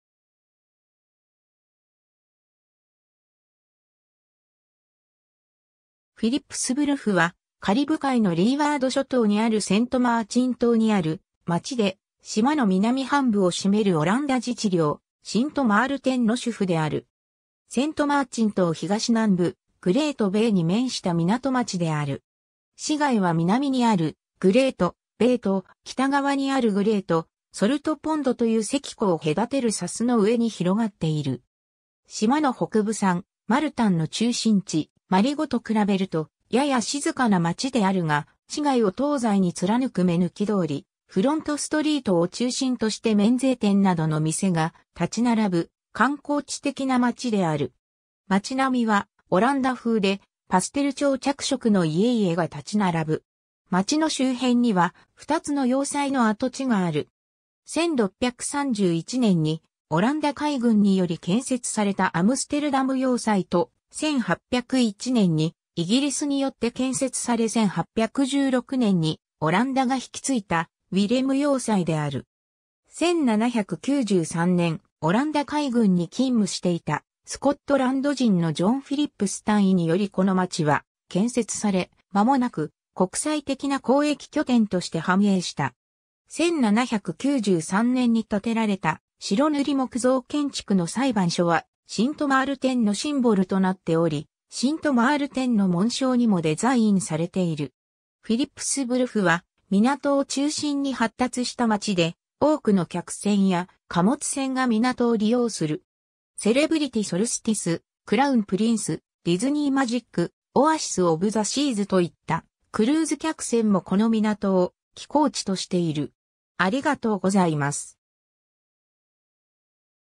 フィリップスブルフ
シント・マールテン
フィリップスブルフ（Philipsburg）は、カリブ海のリーワード諸島にあるセント・マーチン島（シント・マールテン島）にある町で、島の南半部を占めるオランダ自治領シント・マールテンの首府である。
 セント・マーチン島東南部、グレート・ベイに面した港町である。
市街は南にあるグレート・ベイと、北側にあるグレート・ソルト・ポンド (nl:Zoutmeer van de Grote Baai) という潟湖を隔てる砂州の上に広がっている。
 島の北部（フレンチ・サイド）サン・マルタンの中心地マリゴと比べるとやや静かな街であるが、市街を東西に貫く目抜き通り「フロント・ストリート」（Front Street）を中心として免税店などの店が立ち並ぶ、観光地的な町である。
町並みはオランダ風でパステル調着色の家々が立ち並ぶ。
 町の周辺には、2つの要塞の跡地がある。
1631年にオランダ海軍により建設されたアムステルダム要塞 (Fort Amsterdam (Sint Maarten)) と、1801年にイギリスによって建設され1816年にオランダが引き継いたウィレム要塞である。
 1793年、オランダ海軍に勤務していたスコットランド人のジョン・フィリップス（John Philips）大尉によりこの町は建設され、間もなく国際的な交易拠点として繁栄した。
1793年に建てられた白塗り木造建築の裁判所は、シント・マールテンのシンボルとなっており、シント・マールテンの紋章にもデザインされている。
 フィリップスブルフは、港を中心に発達した町で、多くの客船や貨物船が港を利用する。
 「セレブリティ・ソルスティス」「クラウン・プリンス」「ディズニー・マジック」「オアシス・オブ・ザ・シーズ」といったクルーズ客船もこの港を寄港地としている。